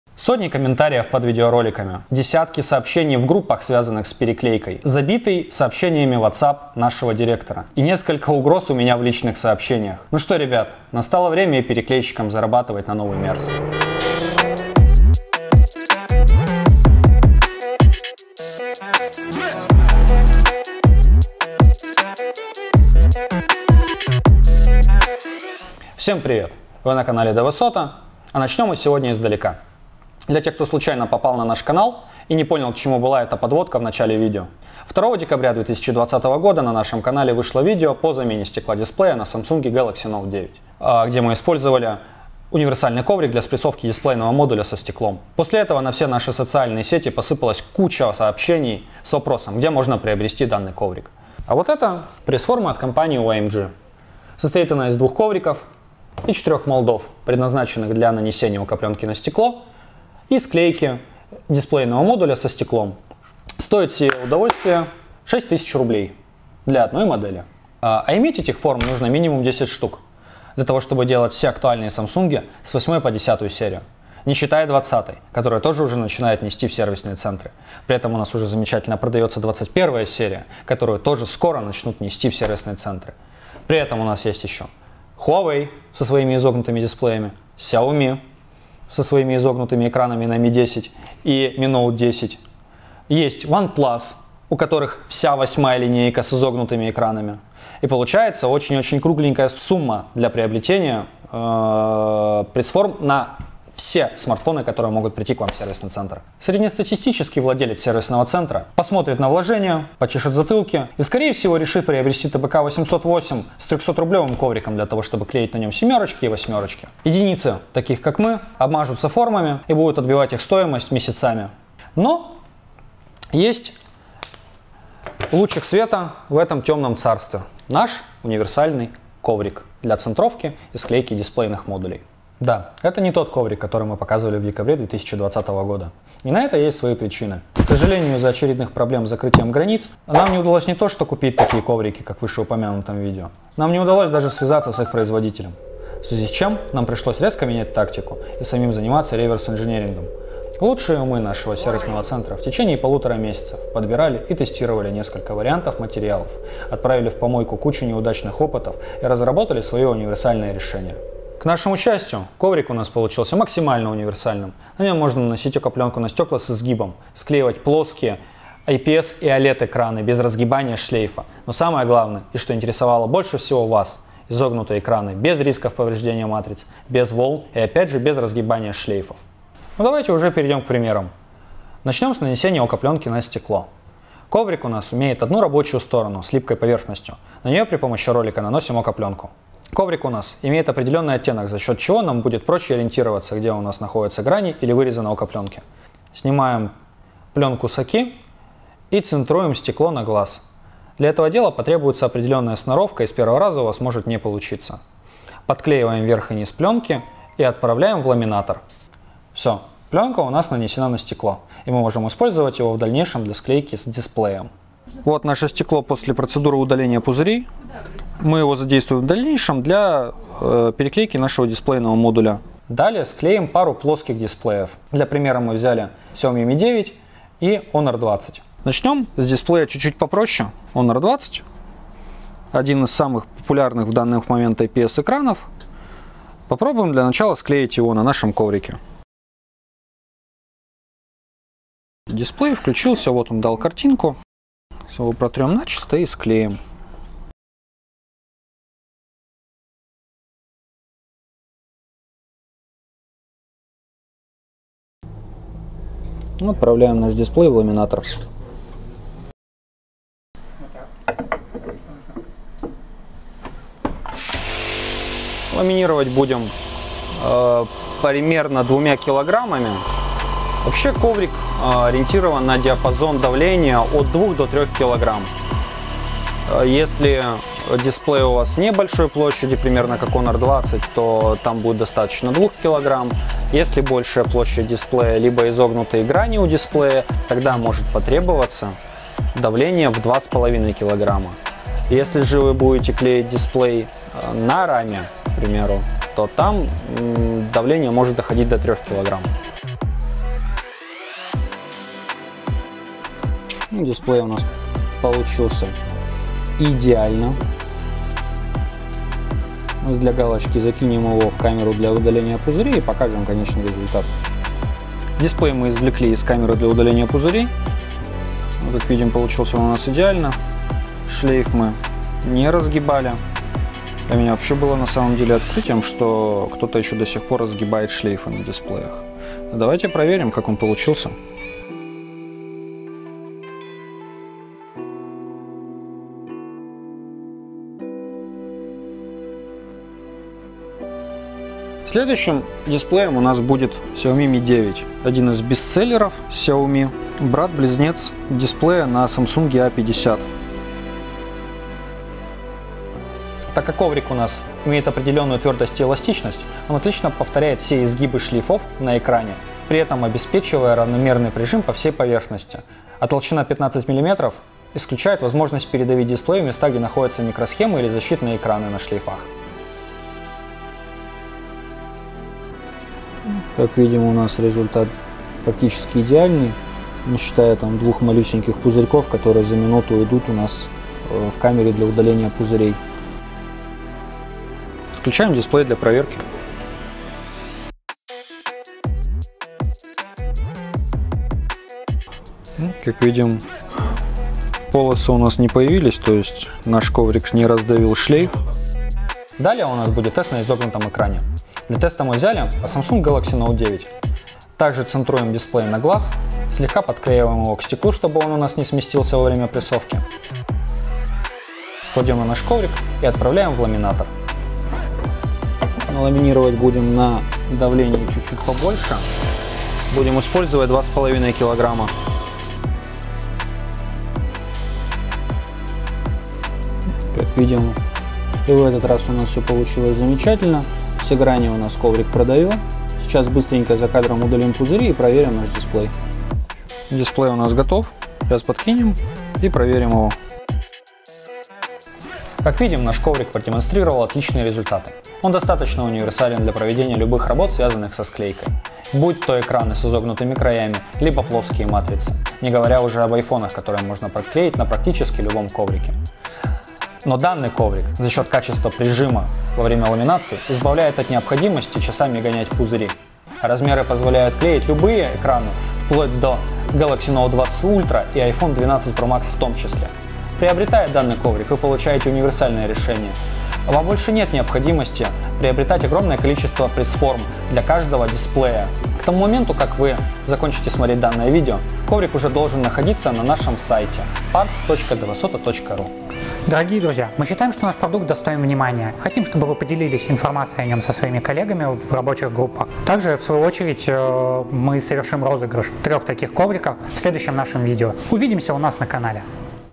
Универсальный коврик для ламинатора под переклейку большинства стекол от СЦ DVSOTA
Уважаемые друзья! Считаю, что наш продукт достоин внимания среди специалистов по переклейке дисплейных модулей и владельцев мастерских и сервисных центров. Помогите распространить данный ролик в группах по ремонту, если посчитаете данный продукт полезным. В следующем, подробном, обучающем ролике  о применении наших ковриков мы выберем трех победителей.

В данном ролике мы расскажем о применении ковриков для ламинатора собственного производства Сервисного Центра DVSOTA.

Универсальный коврик позволяет:

- Нанести ОСА пленку на любое гнутое стекло без специальных форм под стекло ( например Xiaomi Mi Note 10 / Huawei Mate 20 Pro / Huawei P30 Pro / OnePlus 7/ OnePlus 8 и другие

- Произвести склеивание стекла и дисплея как AMOLED дисплеев так и мягких \ гнутых OLED дисплеев без специальных форм